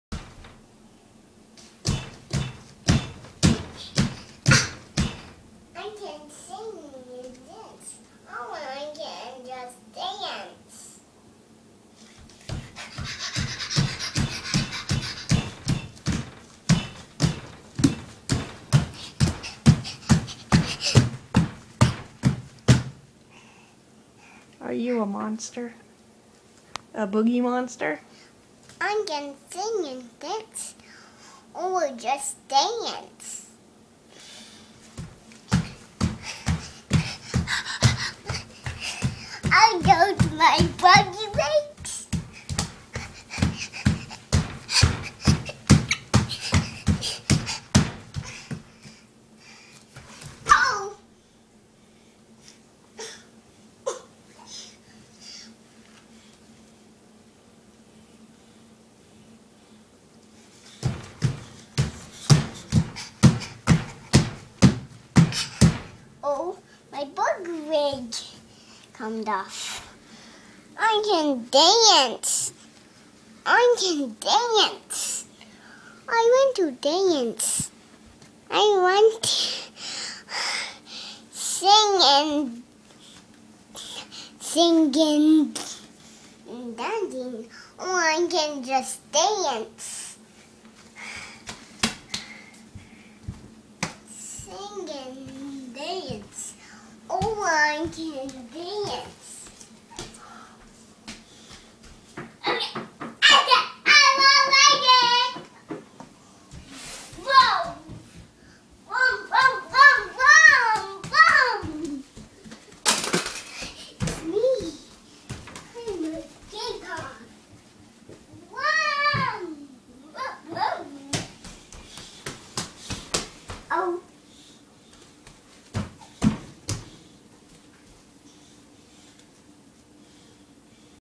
For Gram Gram
Gabriel dug these out and put them on and then asked me to record him.